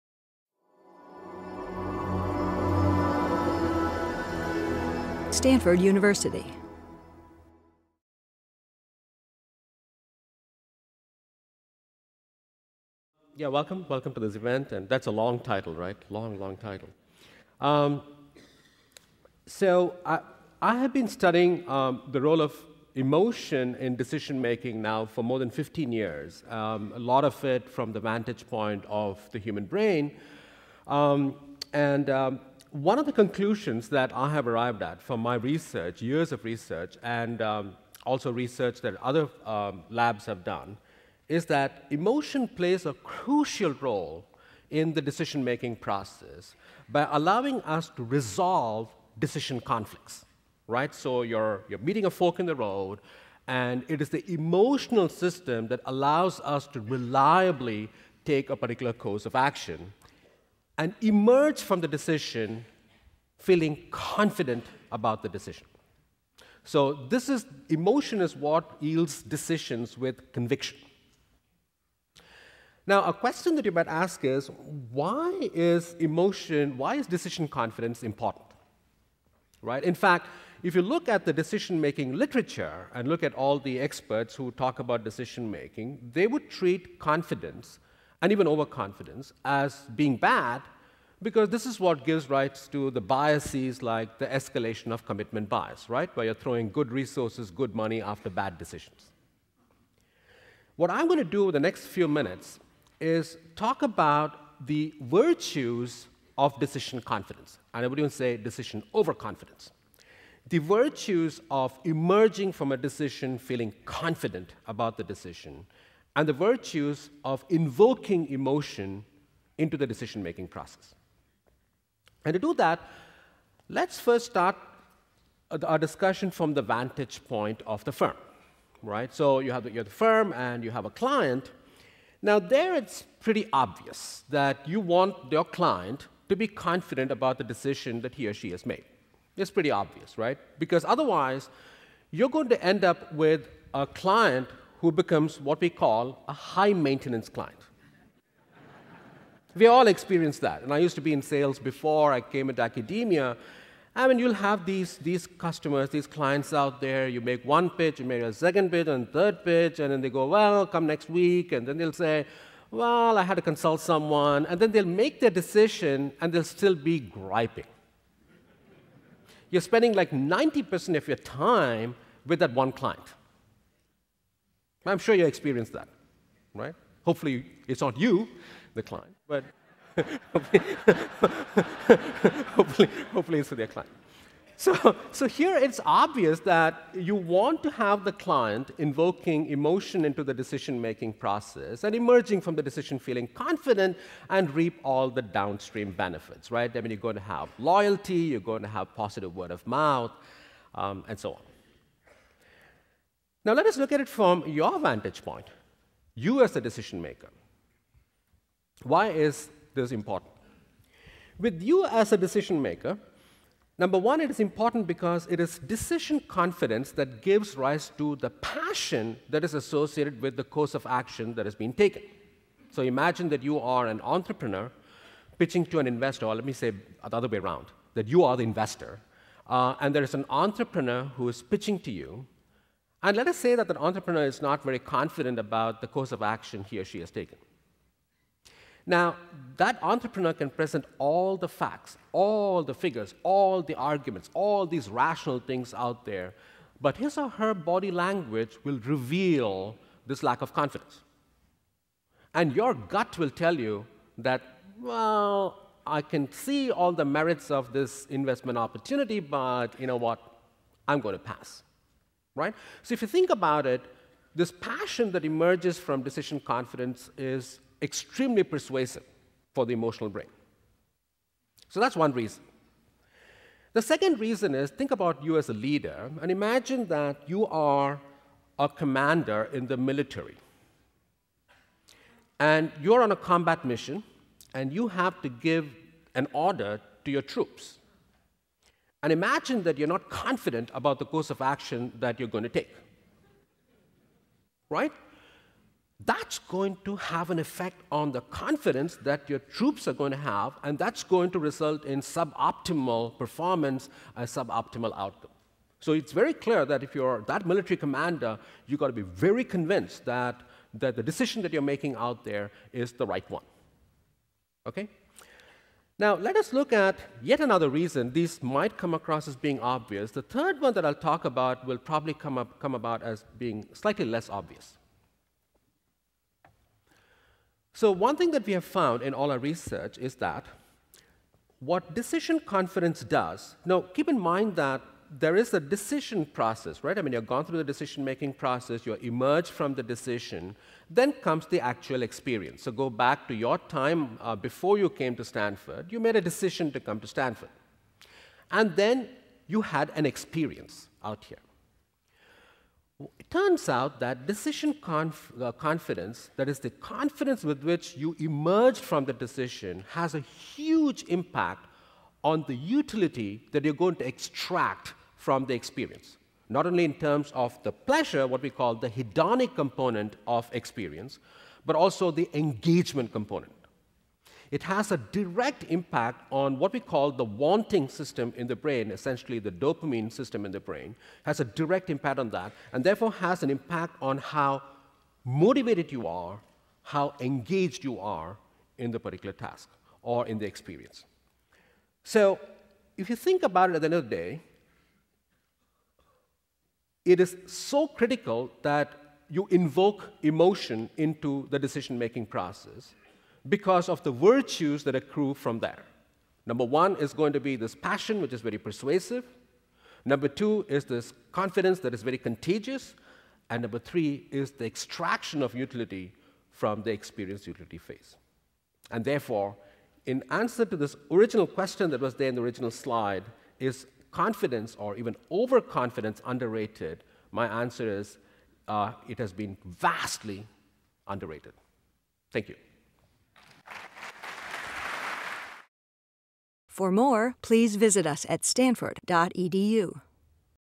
Brain Research at Stanford: Decision Making
October 21, 2011 - Professor Baba Shiv continues the discussion on brain research at Stanford, but takes the topic in a different direction. He focuses on the role that the brain plays in the decision making process that is so fundamental to our existence. He looks at how emotion is interpreted by the brain and ultimately affects the final decision making process.